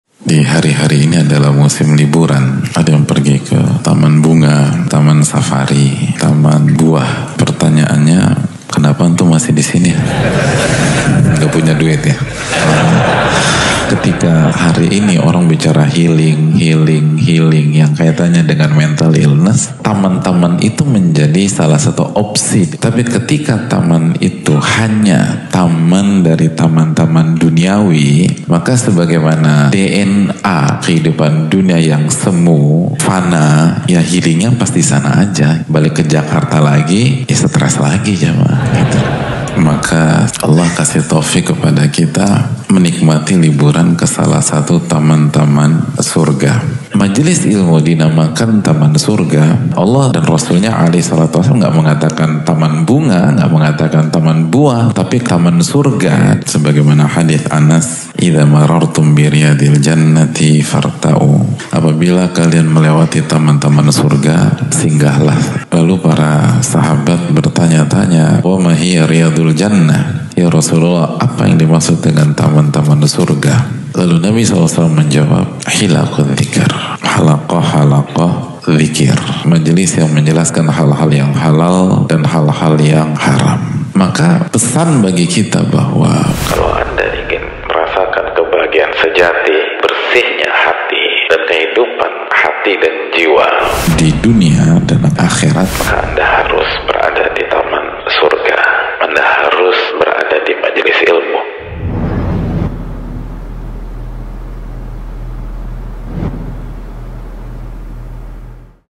HEALING DI 'TAMAN SURGA' | Ustadz Muhammad Nuzul Dzikri hafizhahullah
Video pendek diambil dari Kajian Serial Tadzkiratus Saami' Wal Mutakallim Fii Adabil ’Alim Wal Muta’alim
162 "Doa Menjadi Orang Yang Jujur"